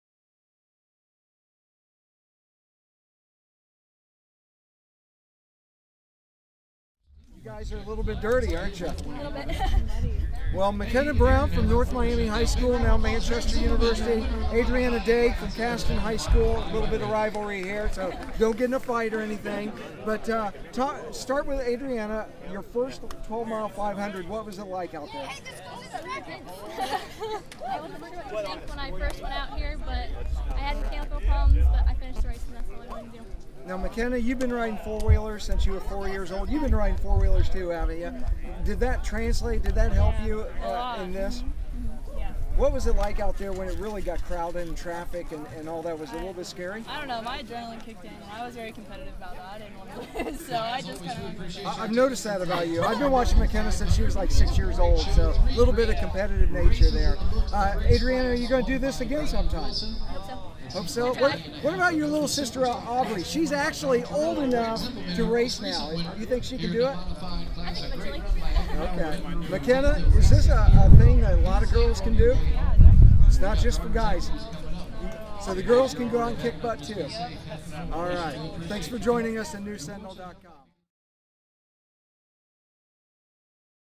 Twelve Mile 500 racers
Makenna Brown and Adrianna Dague speak with News-Sentinel sports editor Tom Davis following their racing in the 52nd annual Twelve Mile 500 Lawnmower Race at Plank Hill Park.